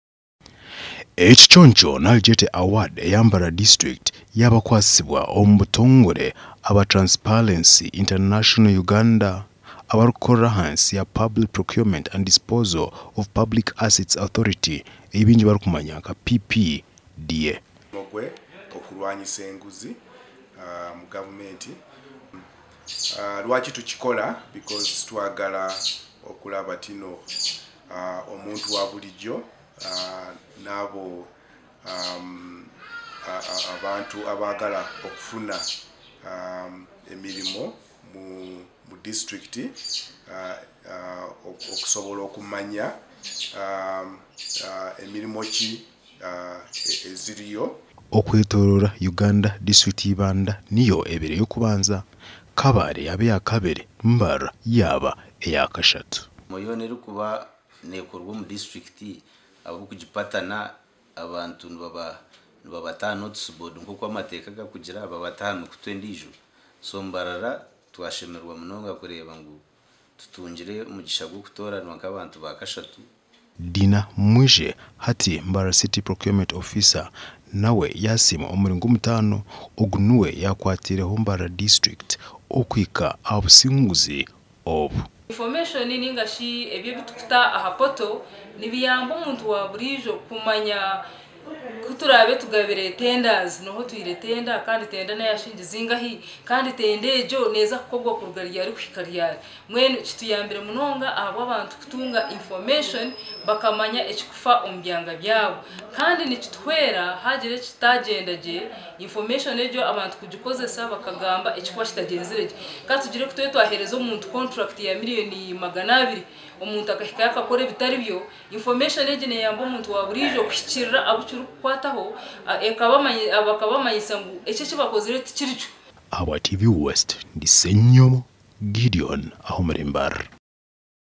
AGACENCWIRE: Okurwanisa oburyarya, disiturikiti ezirikukora gye omu kugura n'okuguza zaasiimwa.
MBARARA:

Okurwanisa oburyarya omu gavumenti z'ebyanga, disiturikiti ezirikukora gye omu kugura n'okuguza zaasiimwa...